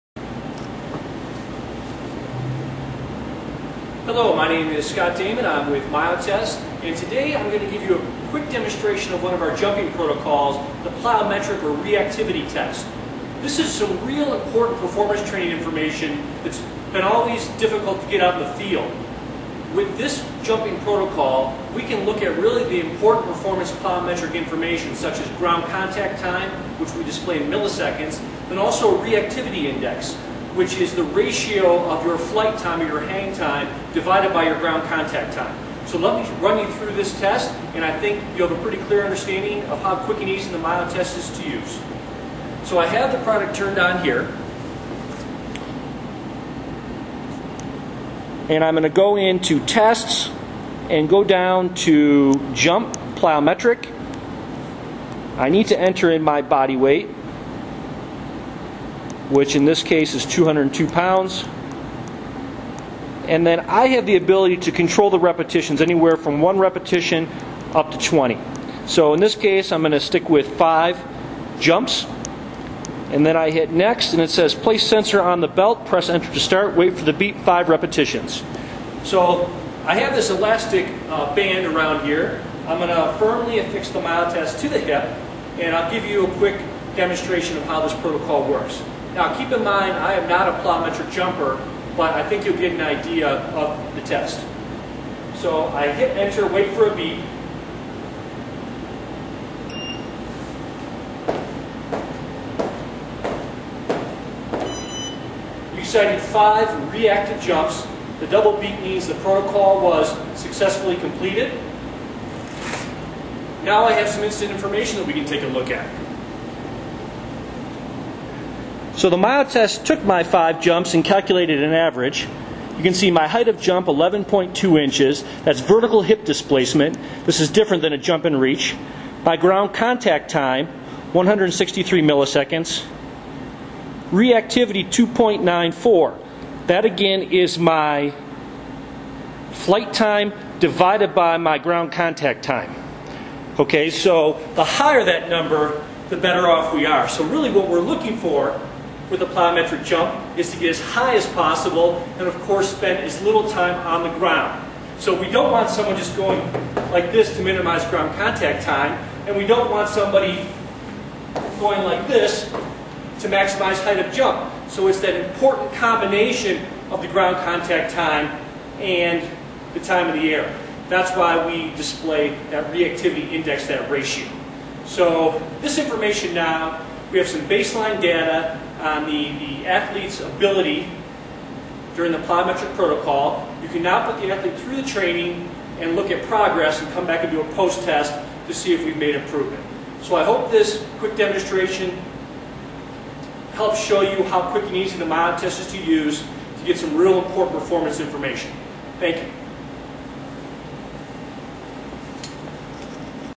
Reactive jump test using the Myotest
Plyometric jump information such as ground contact time and reactivity is measured using the Myotest.  Scott Damman of Myotest explains the assessment protocol and goes through an example.